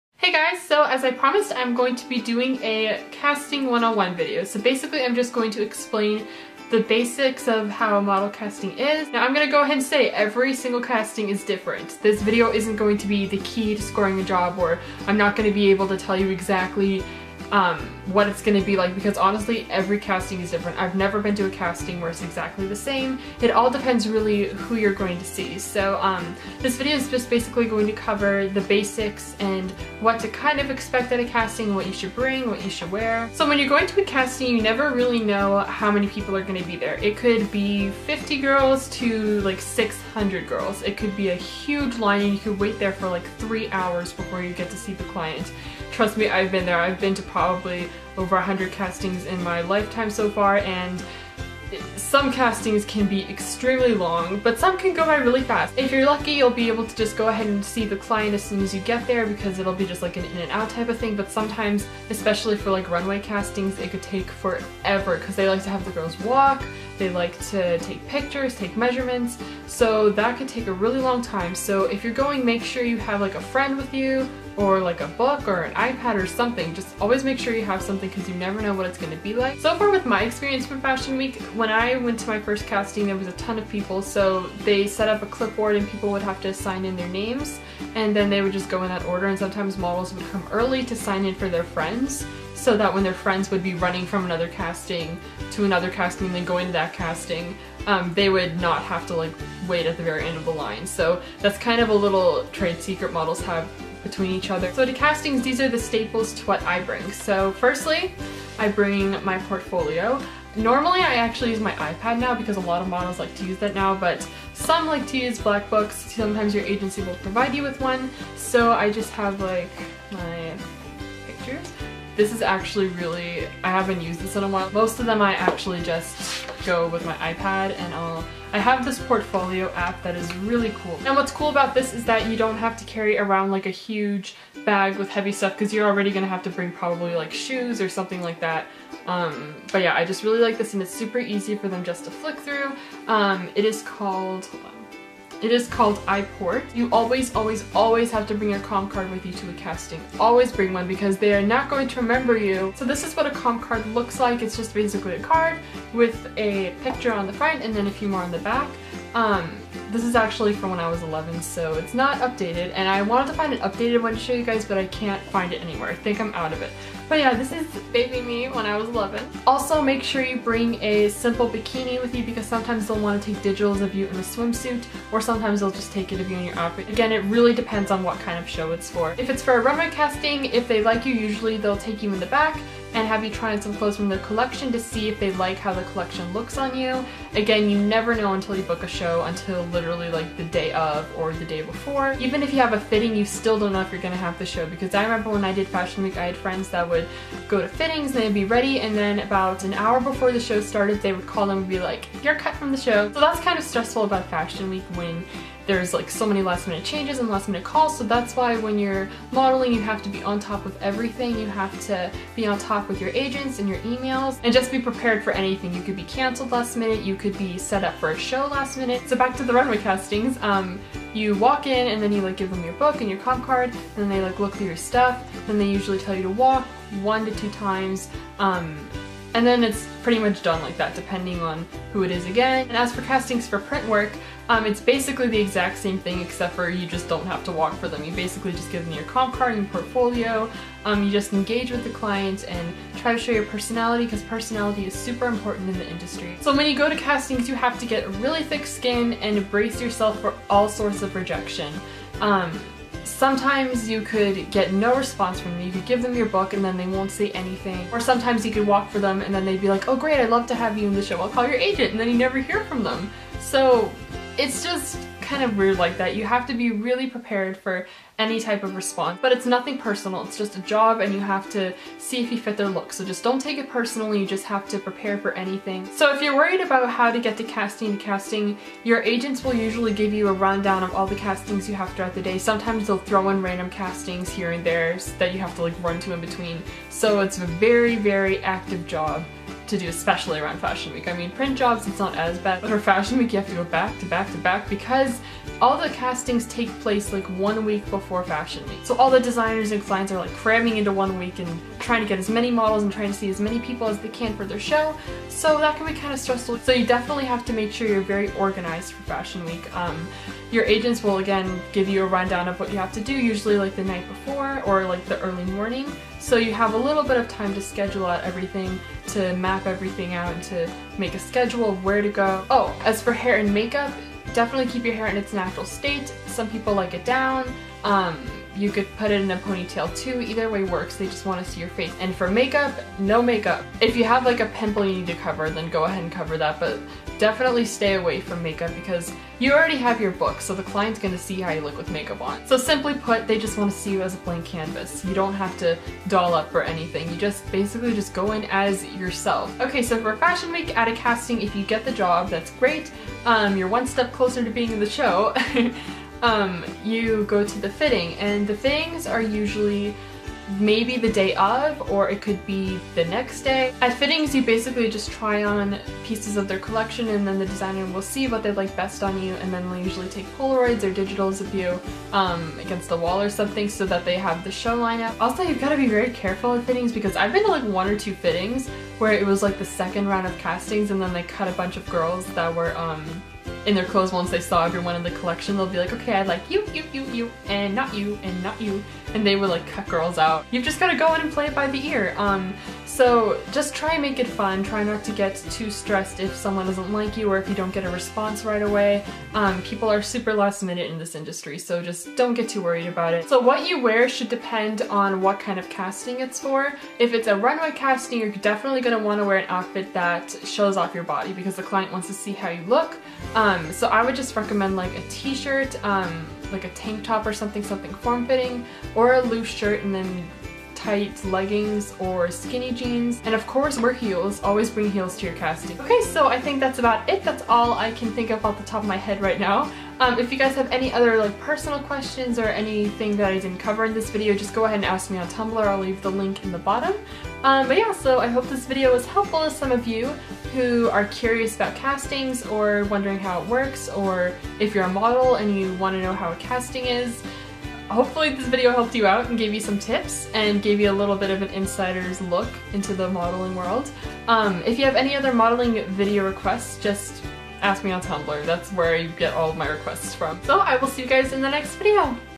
Everon Clark: All About Model Castings
Everon Clarke shares this tips/advice video, on the basis of what all models have learned through their experience in fashion and model industries.